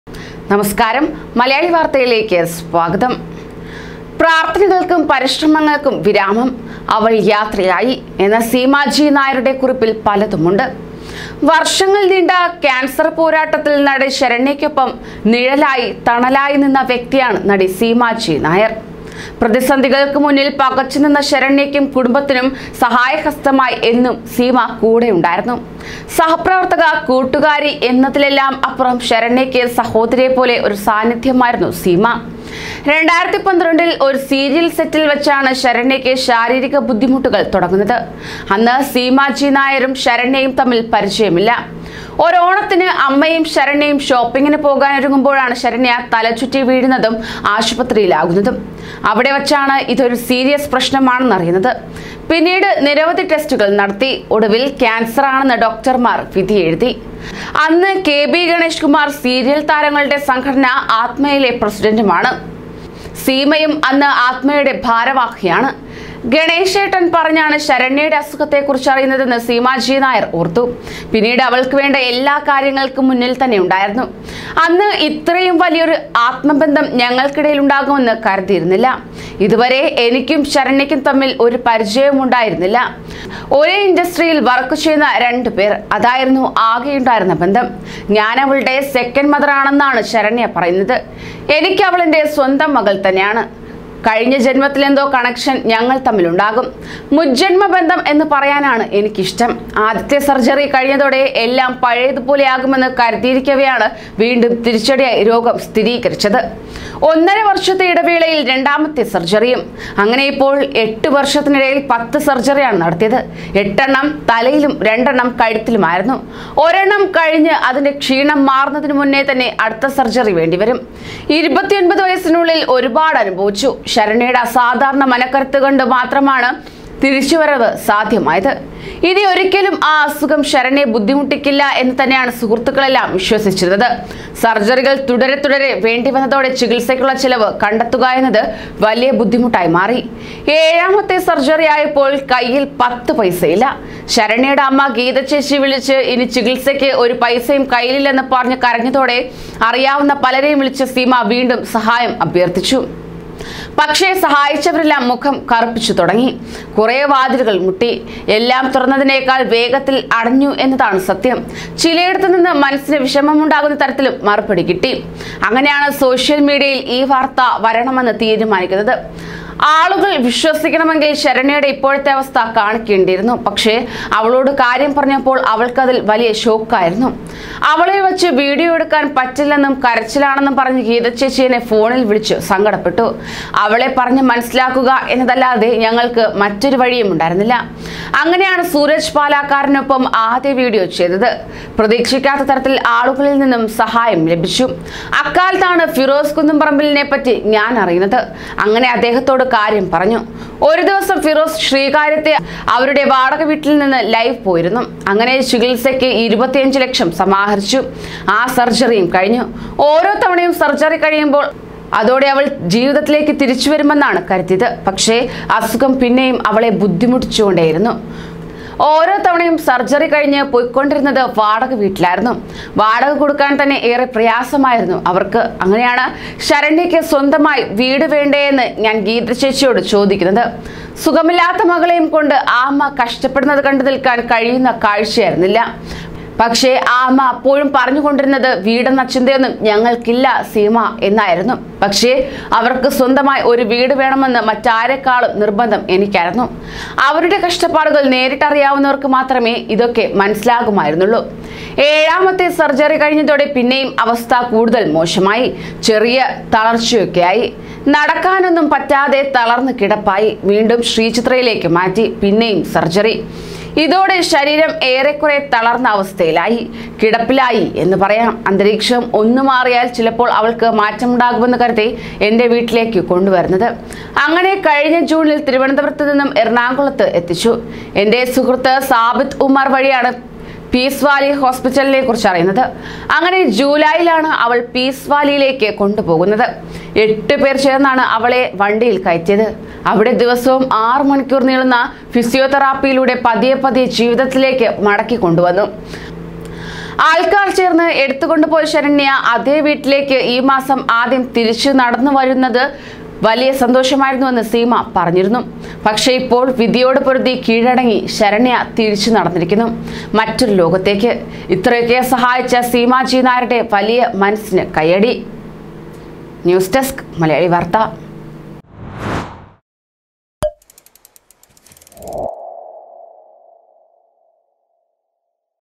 Saranya: എല്ലാം ഒരു സ്വപ്നം പോലെ...
ഗണേഷട്ടന്‍ പറഞ്ഞാണ്
ശരണ്യയുടെ അസുഖത്തെ
കുറിച്ചറിയുന്നത്
പിന്നെ ശരണ്യയുടെ
എല്ലാമെല്ലാമായി
മറക്കില്ല സീമേ ഒരിക്കലും